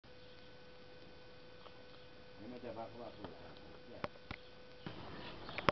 marmelat
Hatice İNCİ erik marmelatı yapıyor.